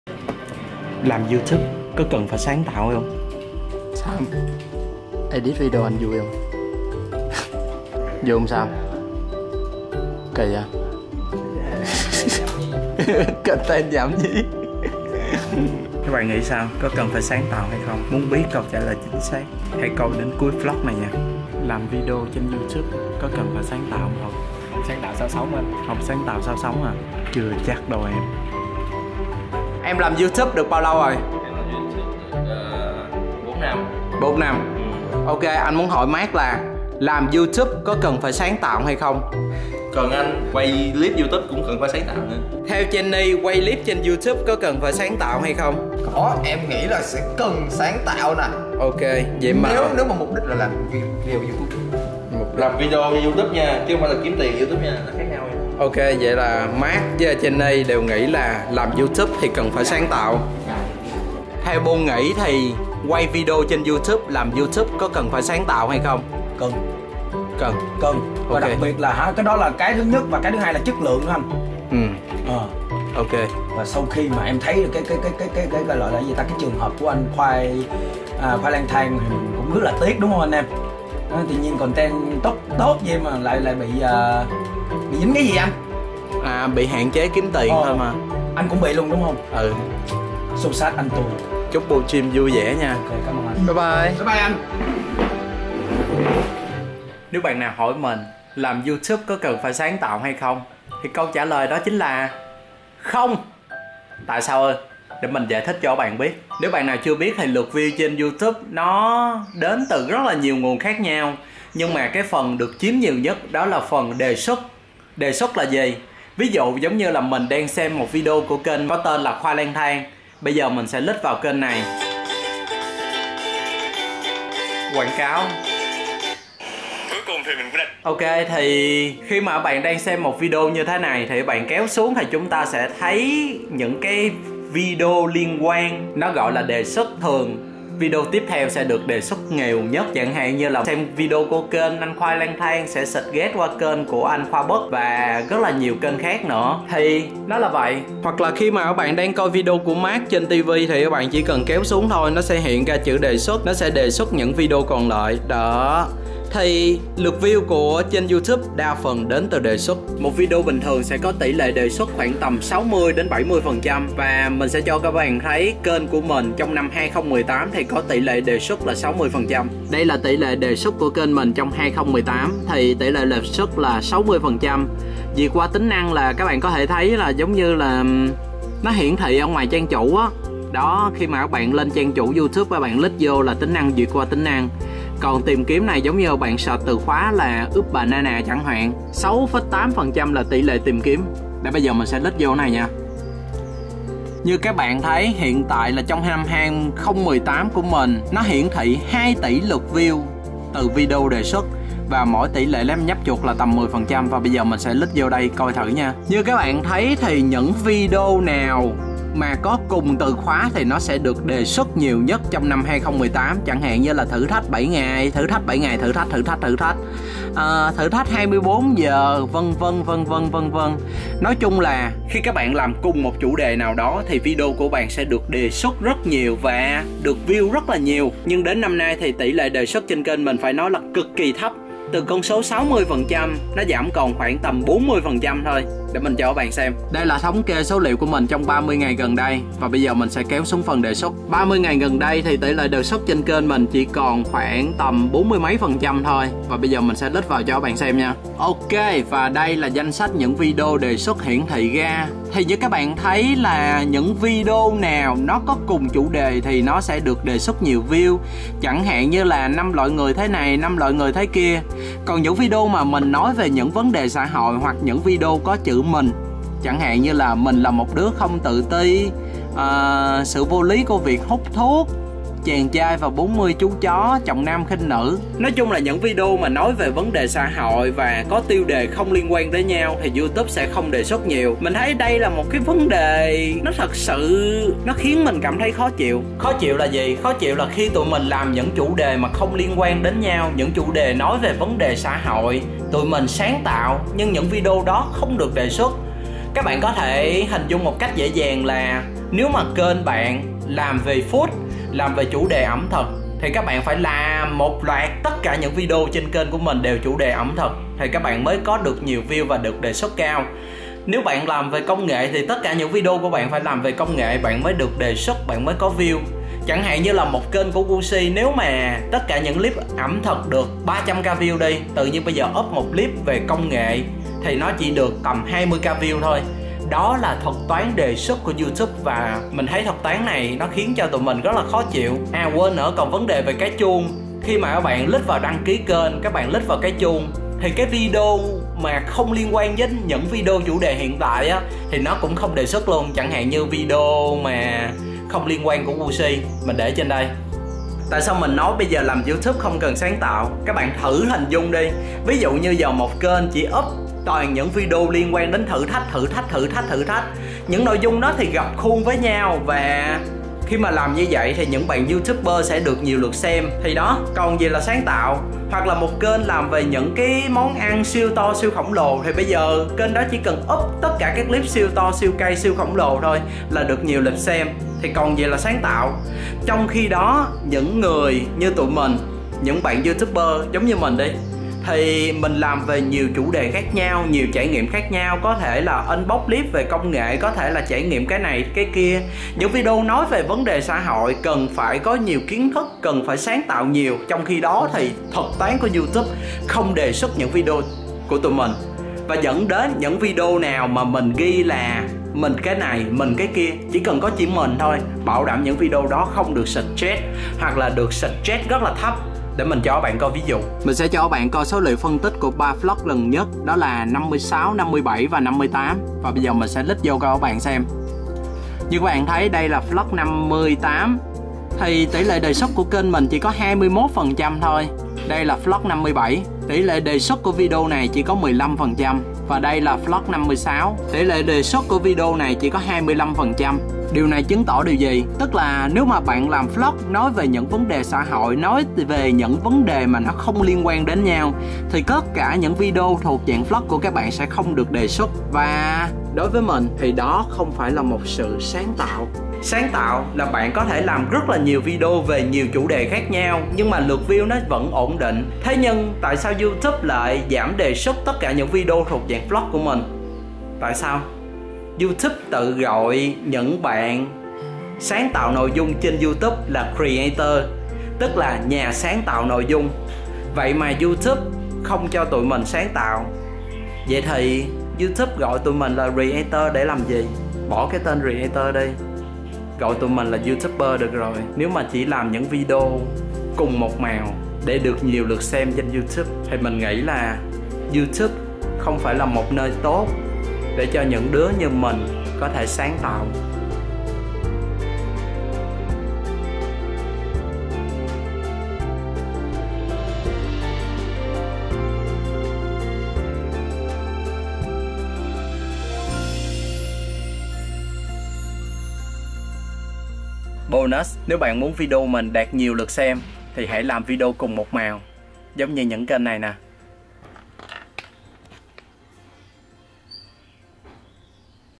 Làm YouTube có cần phải sáng tạo? (Oops Banana Vlog #62)
Mình biết có nhiều bạn muốn làm YouTuber, nhưng thật ra sau một thời gian làm YouTube thì mình thấy thuật toán đề xuất view không còn phù hợp nữa.